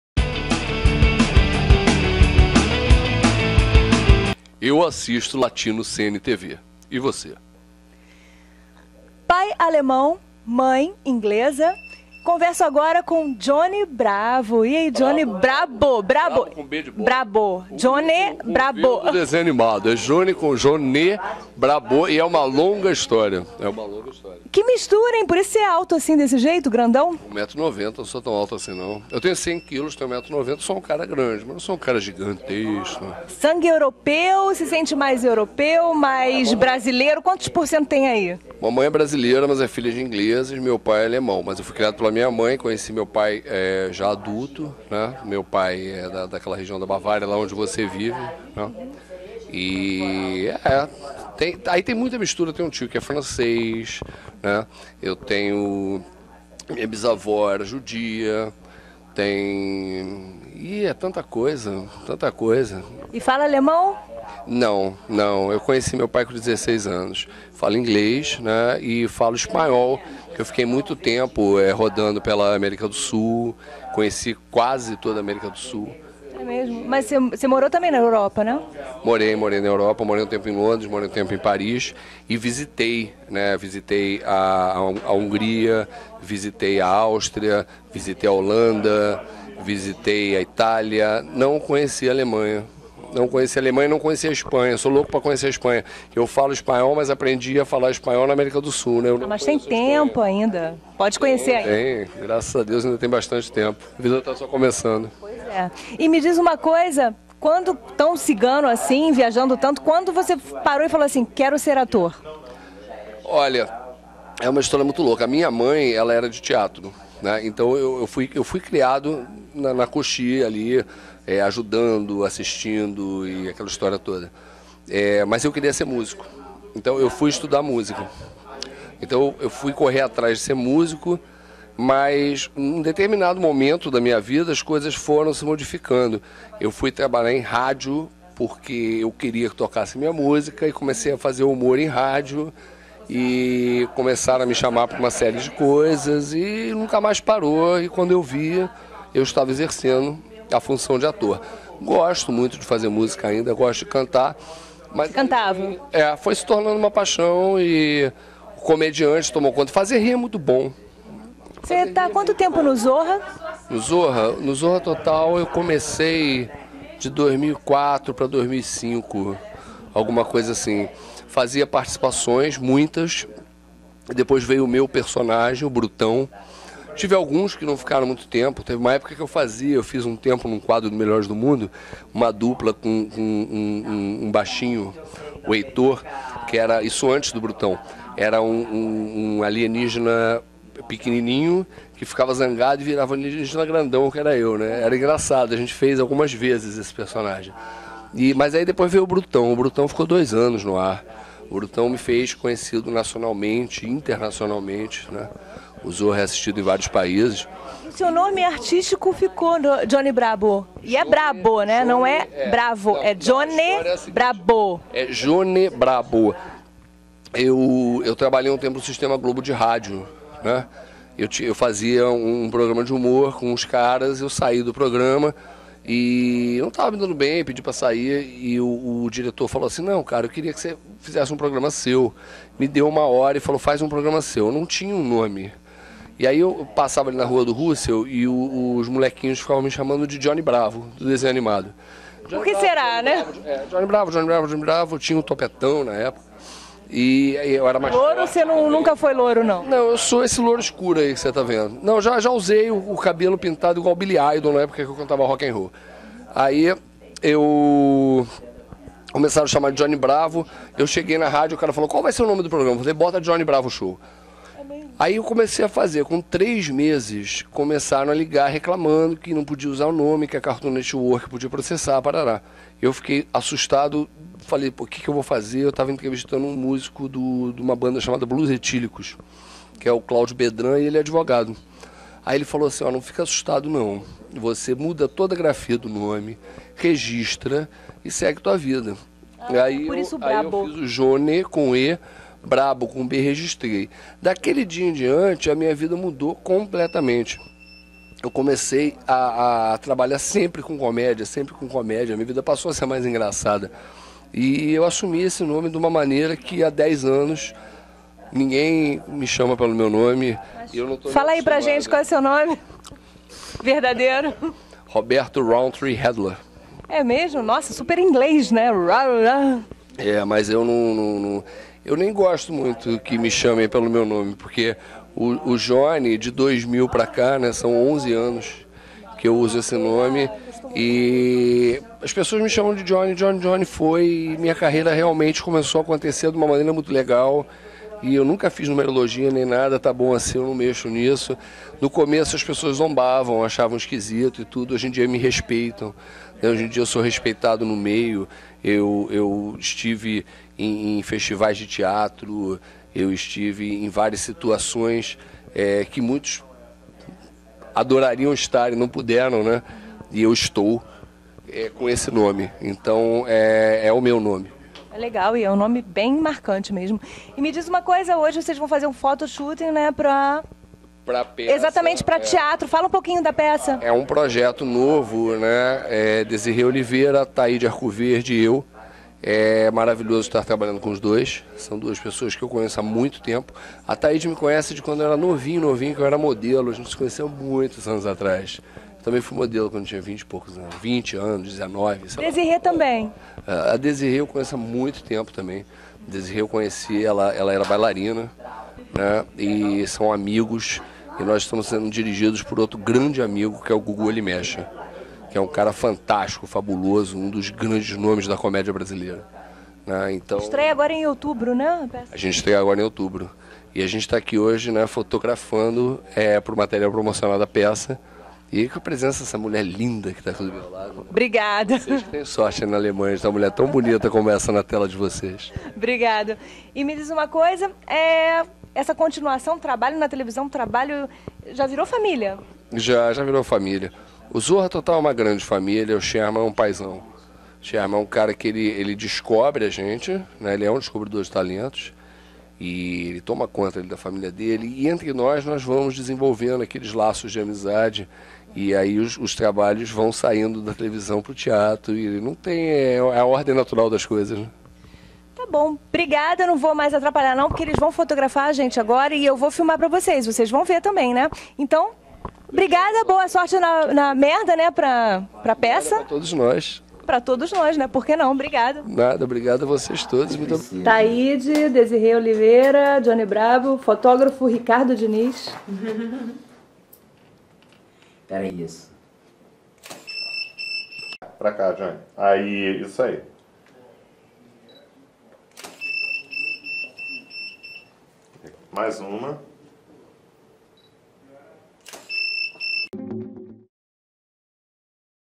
Adriana Calado entrevista Jone Brabo!
Jone Brabo faz um fotoshooting para divulgar seu espetaculo com seus colegas de elenco. Adriana Calado acompanha a sessao de fotos e entrevista o ator, que fala de sua carreira, como comecou, revela seu nome de batismo e conta como chegou ao sucesso. Isso e muito mais...confira!